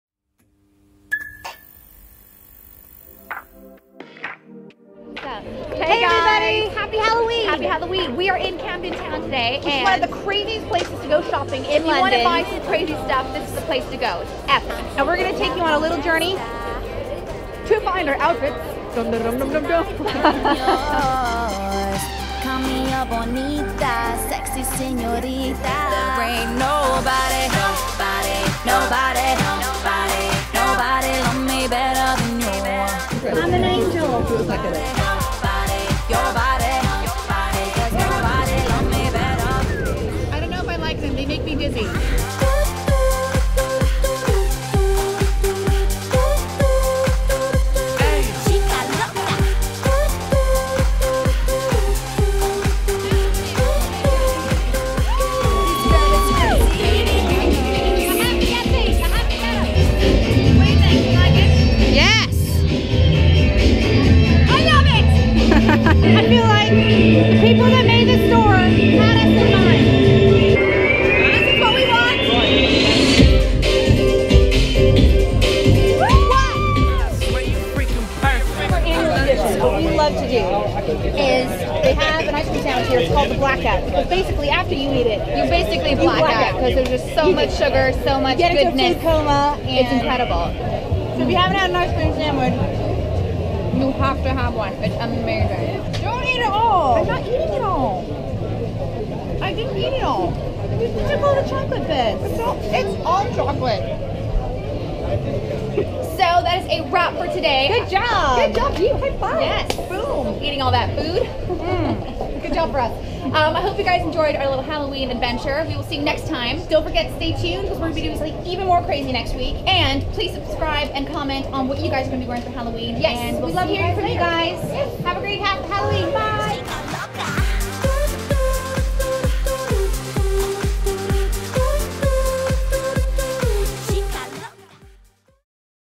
Halloween Costume Fun Part 1
Join Jazzy and Ruby on an adventure round Camden Town and its market searching for the perfect halloween costume for fright night!
Ep.2 Part 1